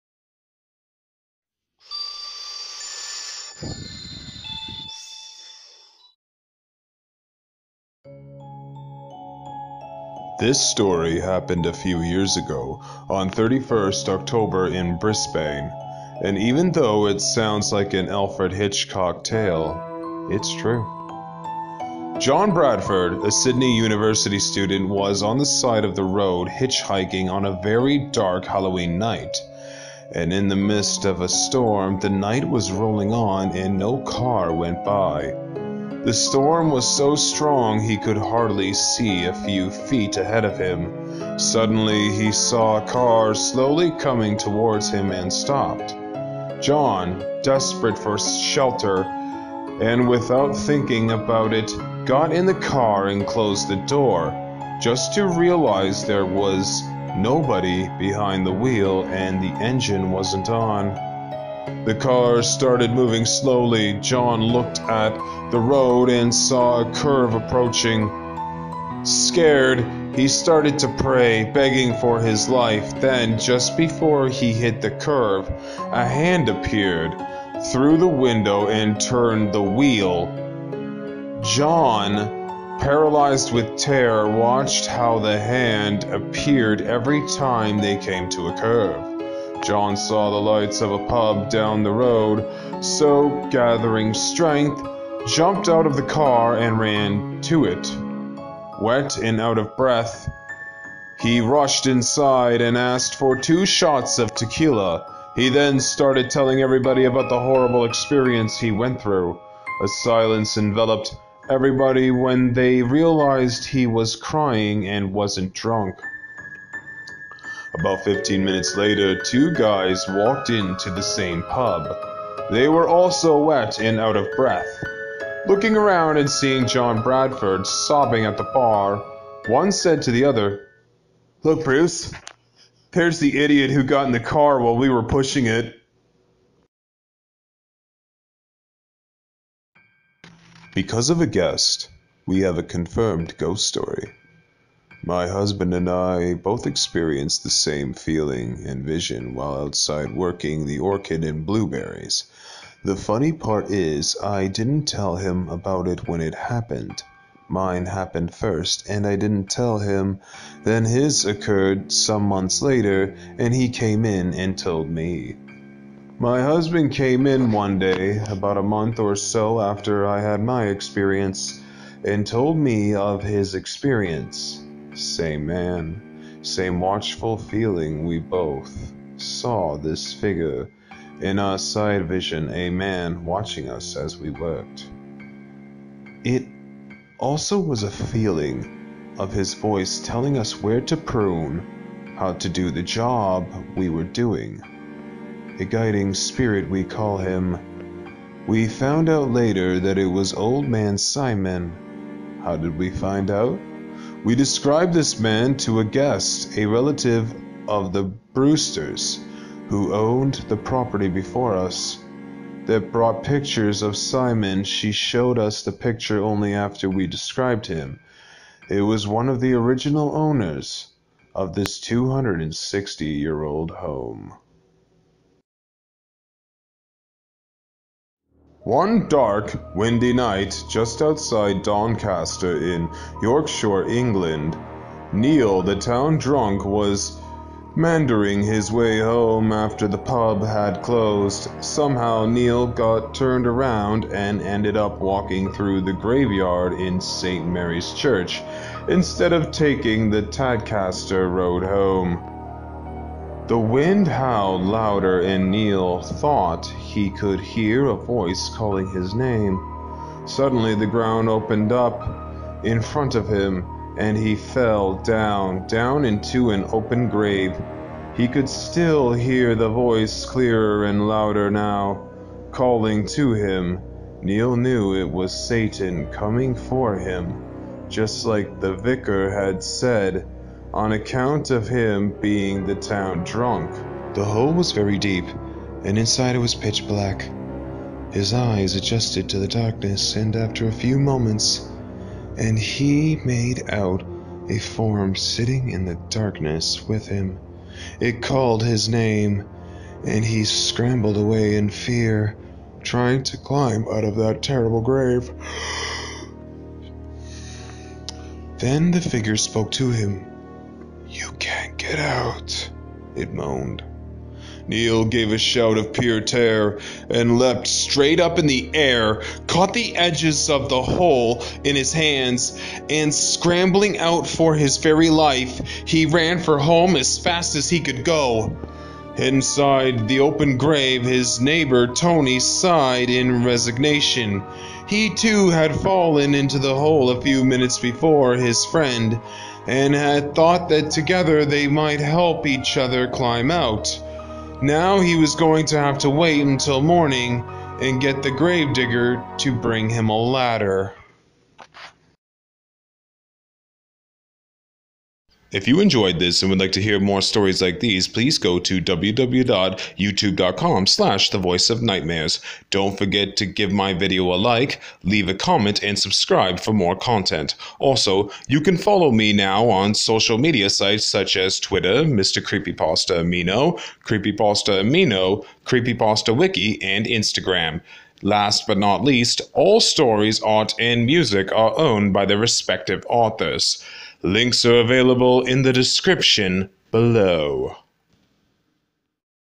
Halloween stories from the web
These are a bunch of anti climatic horror stories. Found on some website. Now, I know it says funny, however, they're are mostly anti climatic. The reason why I chose these, is due to me, wanting to ease up some of my depression.